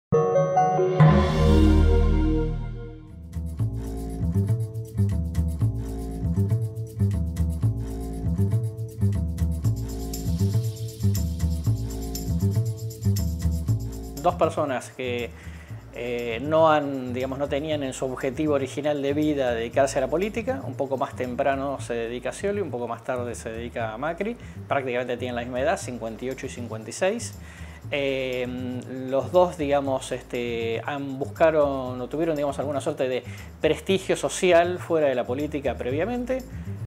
Scioli y Macri: dos aspirantes presidenciales con orígenes
La legisladora porteña Graciela Ocaña y los analistas políticos Rosendo Fraga y Carlos Fara opinaron ante DyN sobre una particularidad que comparten el gobernador bonaerense y el jefe de gobierno porteño: ambos se incorporaron a la política partidaria luego de desarrollarse en el mundo empresarial y del deporte.